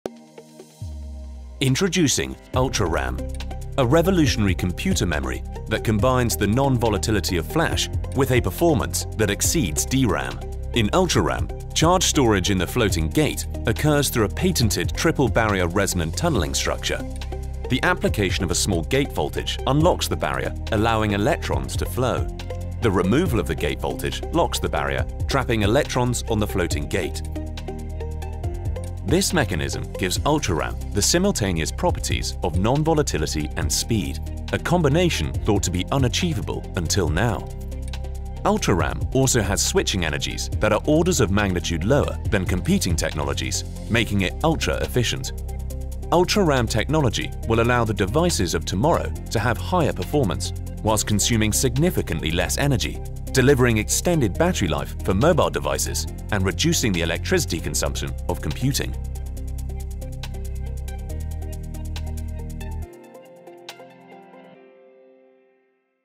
Introducing ULTRARAM - a revolutionary computer memory
ULTRARAM is a revolutionary computer memory that combines the advantageous properties of flash and DRAM into a single memory concept. It uses quantum mechanics and a process called 'resonant tunnelling' to achieve these remarkable properties. Applications include: space, quantum computing, defence, IoT, cloud computing, data centres and consumer electronics such as PCs and mobile phones.